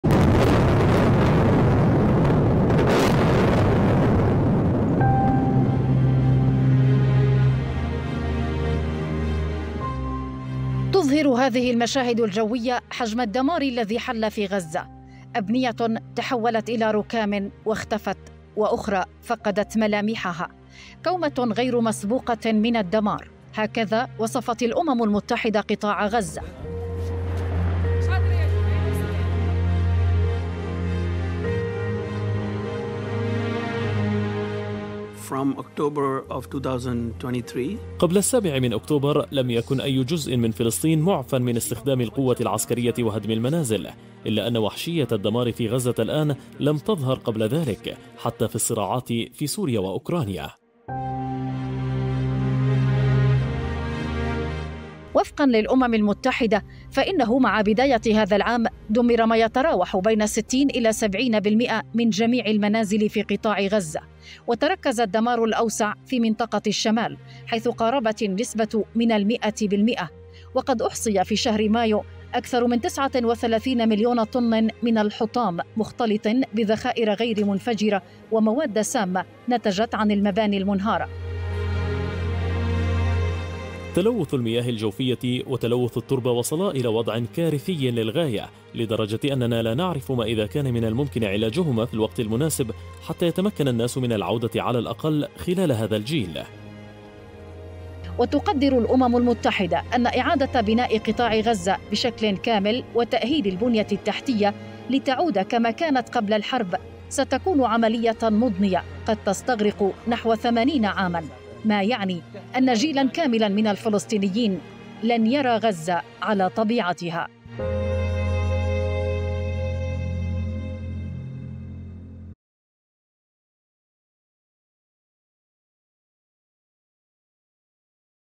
غزة تحتاج 80 عاماً لتعود كما كانت.. الأمم المتحدة تكشف إحصائيات الدمار
"إعادة إعماره بشكل كامل قد تستغرق 80 عاماً".. الأمم المتحدة تكشف عن إحصائيات حول الدمار في قطاع غزة